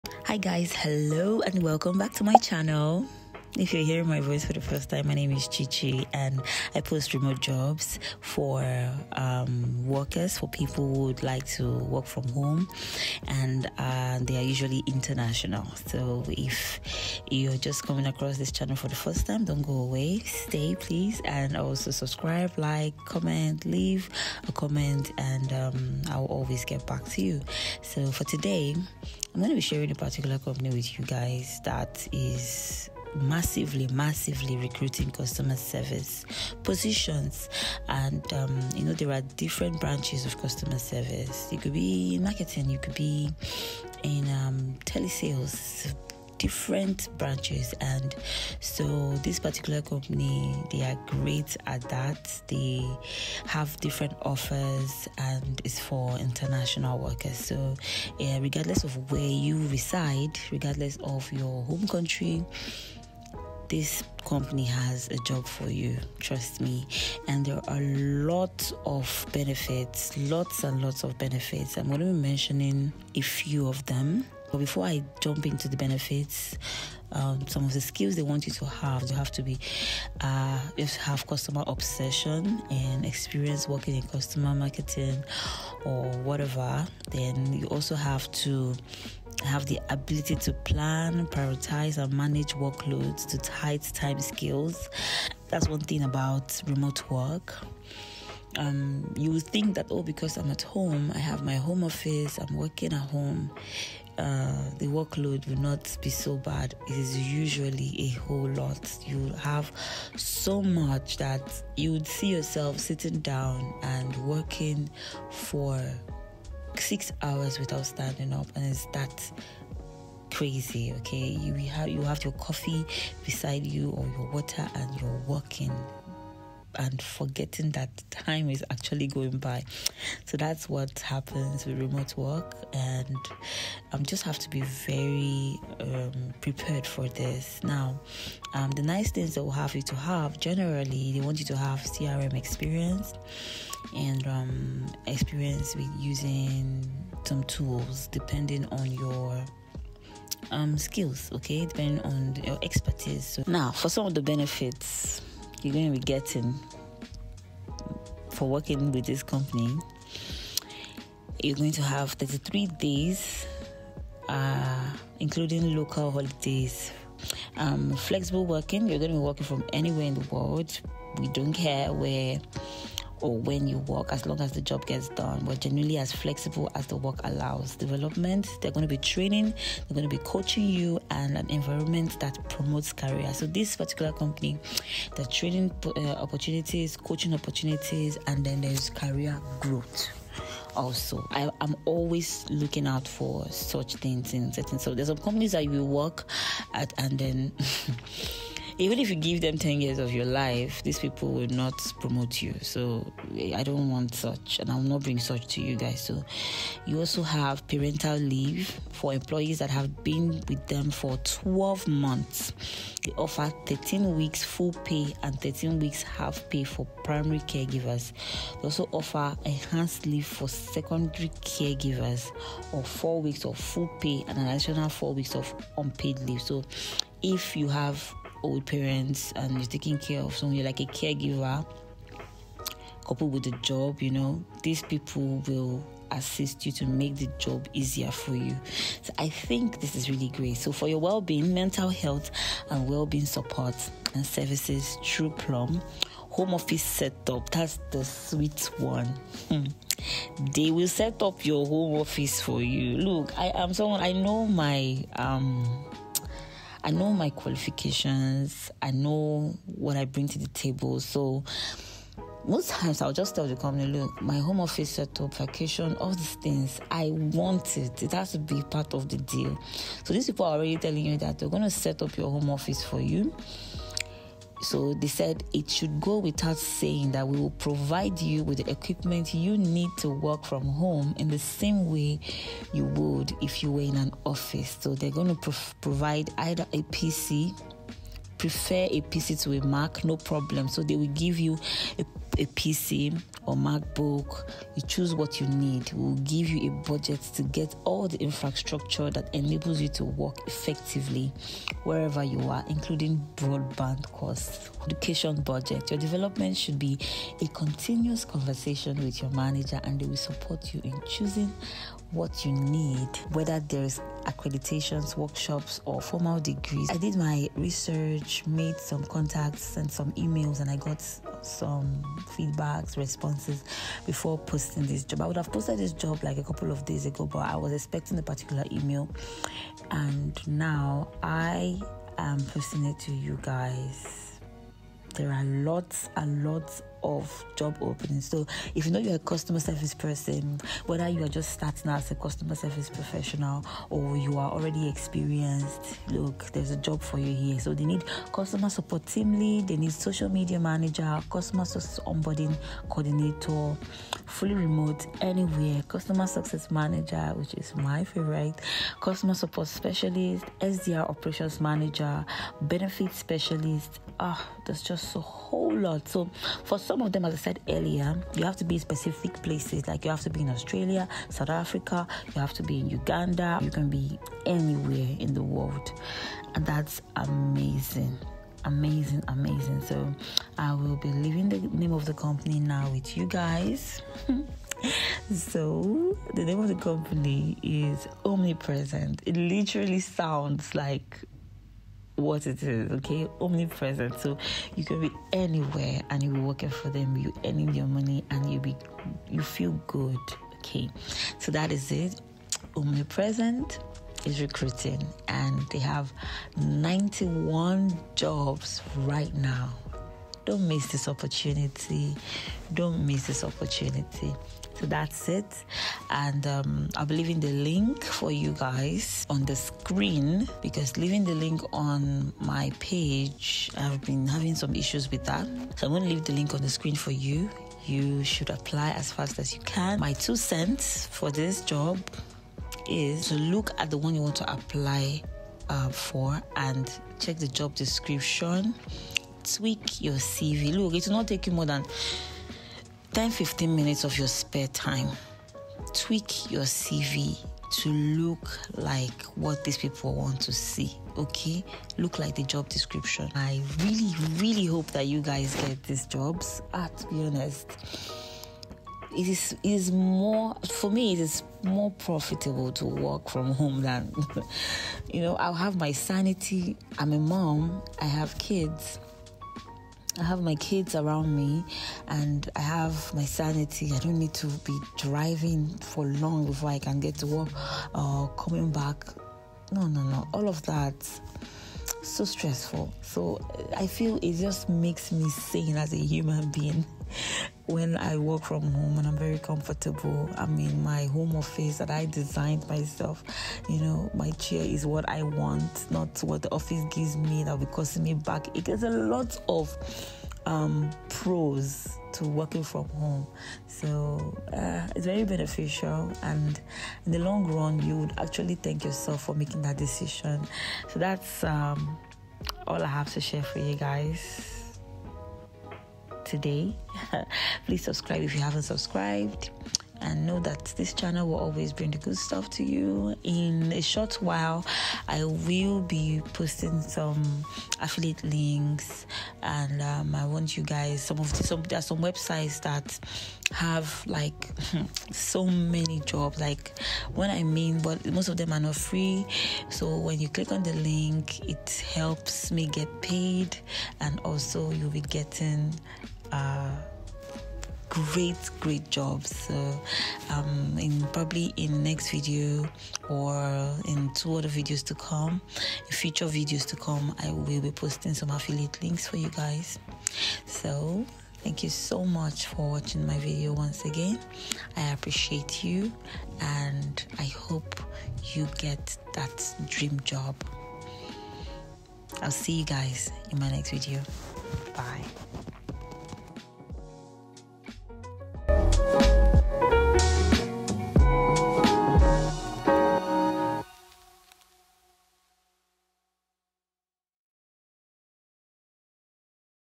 Top International Remote Jobs for Africans, Middle-Easterners, Asians and South/North Americans.
Hey People, In this video I shared an amazing company where you can find international remote jobs for workers in Africa, the middle East, Asia and the Americas. I also shared tips on how to get instantly noticed by the companies/recruiters hiring. work from home jobs 2022 virtual assistant gig economy cici dawn latest jobs